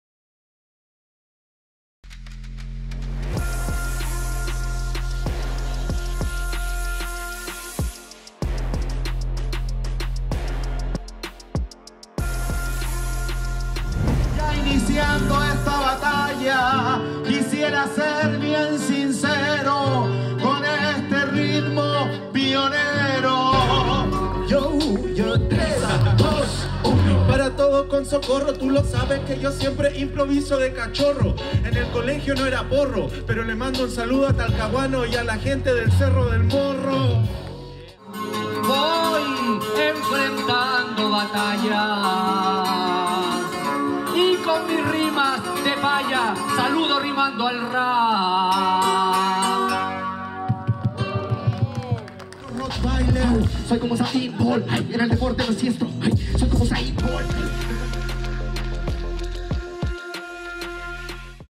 Encuentro de Payadores y Freestylers en Lo Barnechea
Manuel Sánchez y Luciando Auad en la paya y Jokker y Tom Crowley en el freestyle.